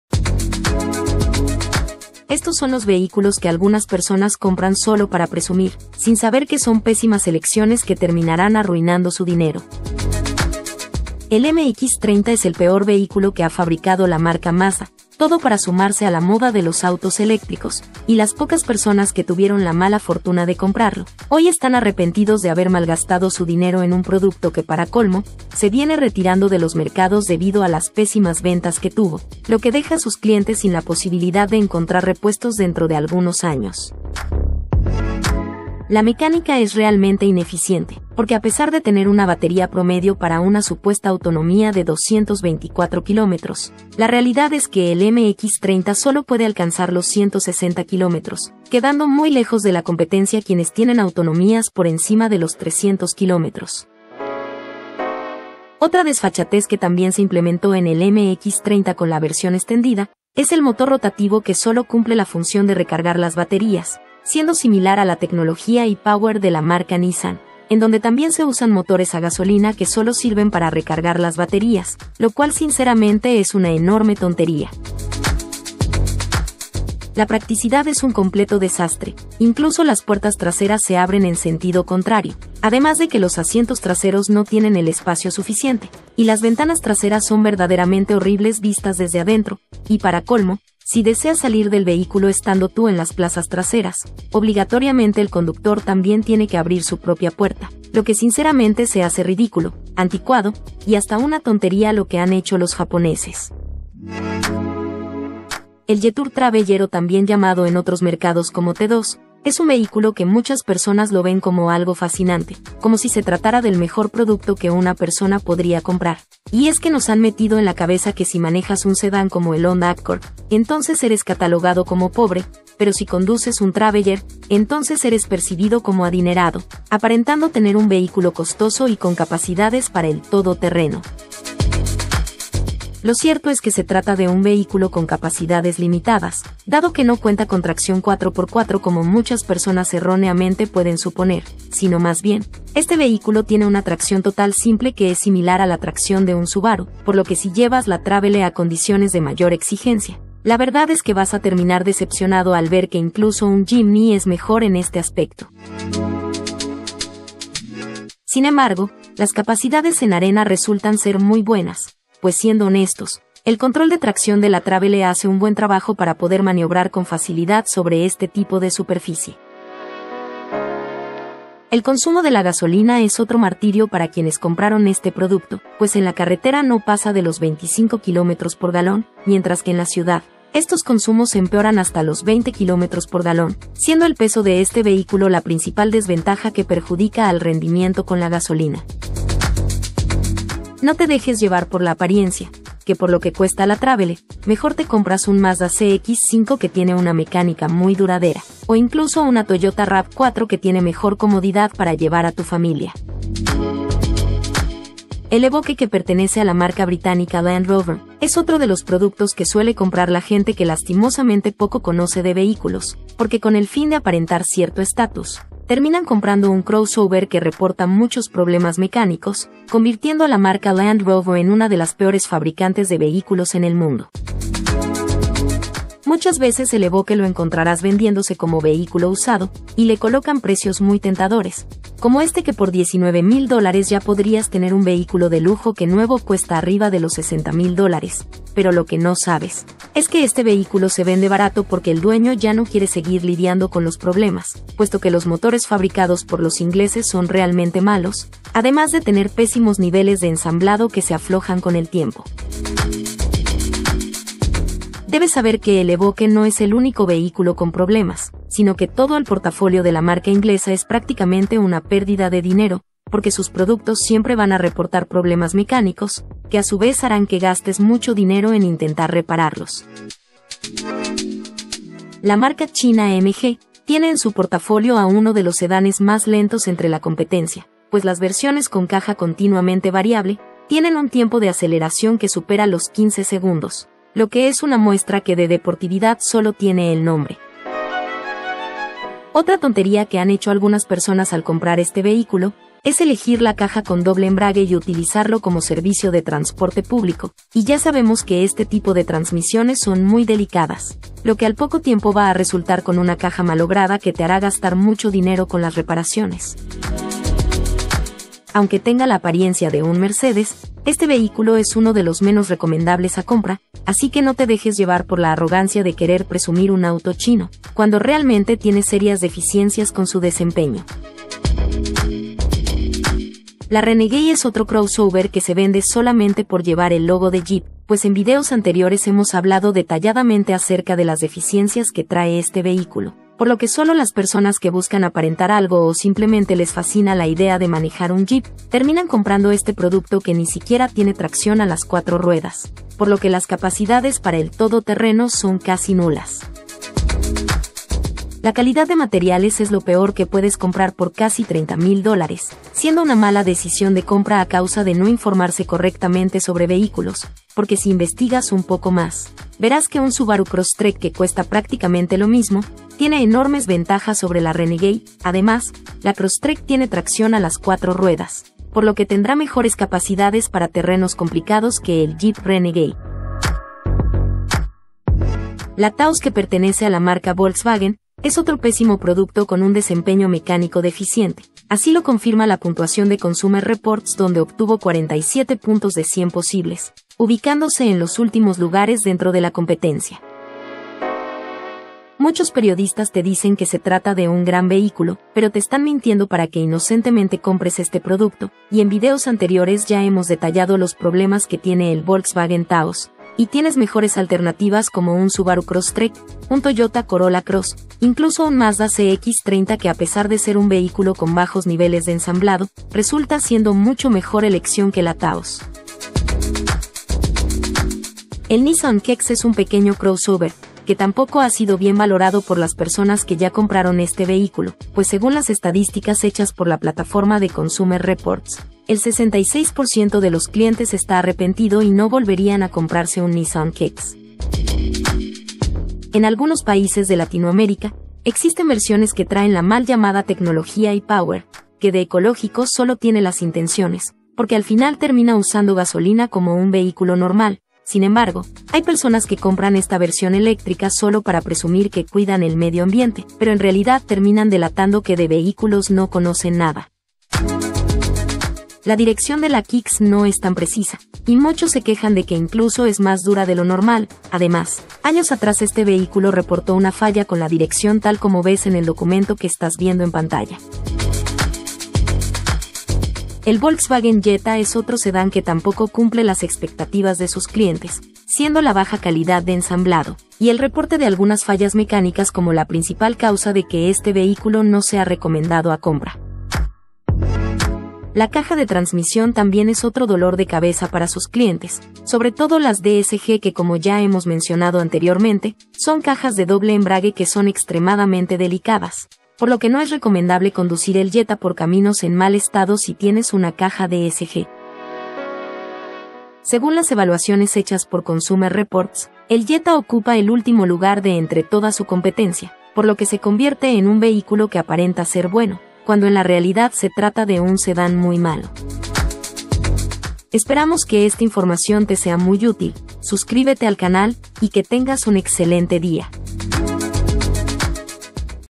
Solo los TONTOS compran estos VEHÍCULOS
Estos son los vehículos que algunas personas compran solo para presumir, sin saber que son pésimas elecciones que terminarán arruinando su dinero.

0:00 INTRO
0:12 MAZDA - MX 30
1:46 JETOUR - TRAVELLER / T2
3:26 LAND ROVER - RANGE ROVER EVOQUE
4:33 MG - GT
5:24 JEEP - RENEGADE
6:16 VOLKSWAGEN - TAOS
6:59 NISSAN - KICKS
7:54 VOLKSWAGEN JETTA